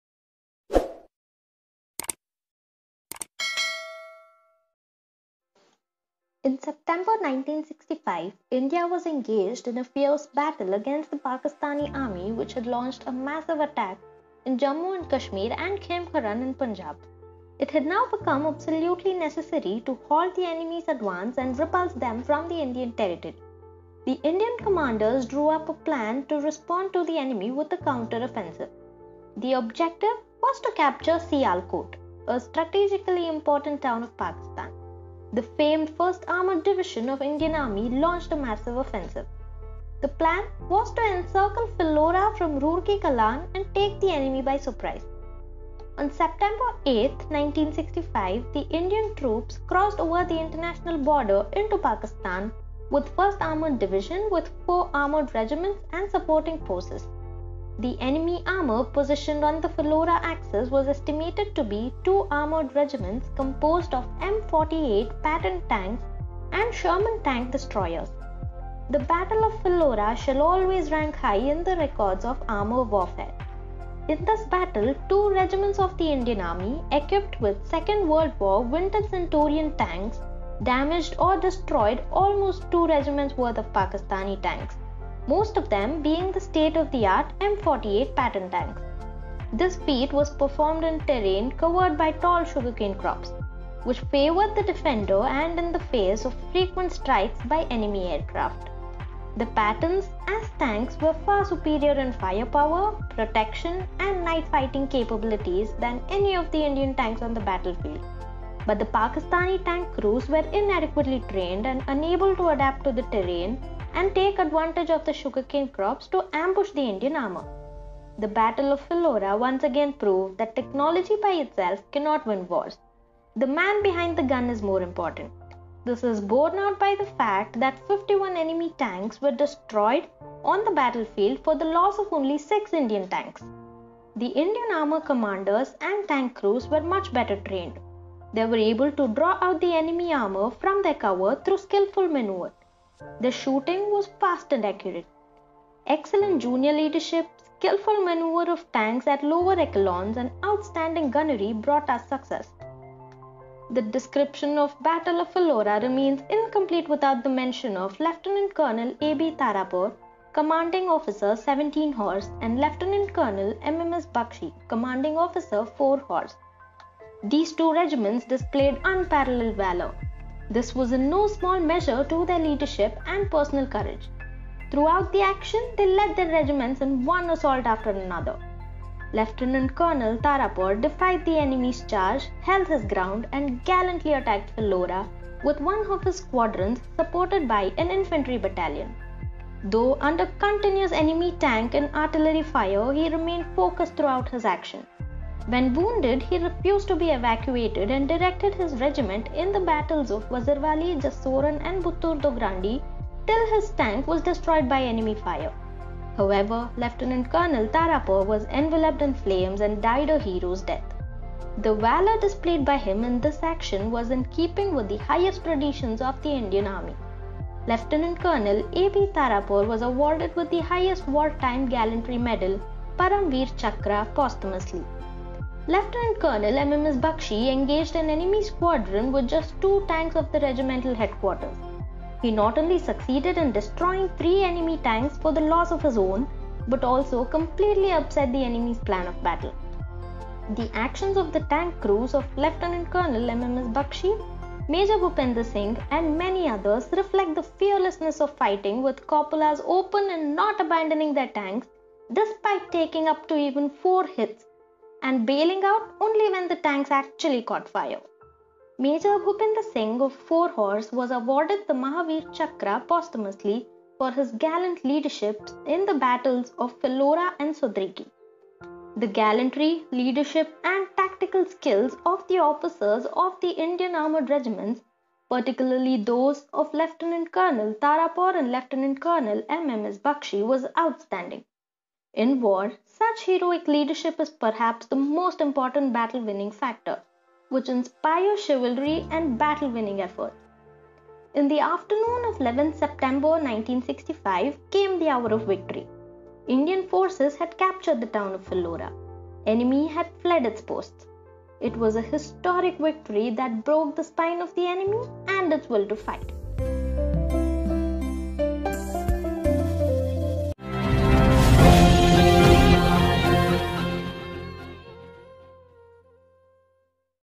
The Battle of Phillora 1965 | ಪಿಲ್ಲೋರಾ ಯುದ್ಧ | Veera Yodhara Samaragathe | Day #10
The Battle of Phillora

The Battle of Phillora was among one of the largest tank battle fought between India and Pakistan during 1965 Indo-Pak war. In this battle Pakistan was on the better side because it was being supported by America which provided it with Patton tanks because of cold war politics which was going on whereas India did not have such weapons.
This battle is remembered because of how India with inferior number of tanks and inspired leadership won against Pakistan with superior number in tank strength and arms support from mighty nations.

DO FOLLOW OUR CAUSE:-

Source:-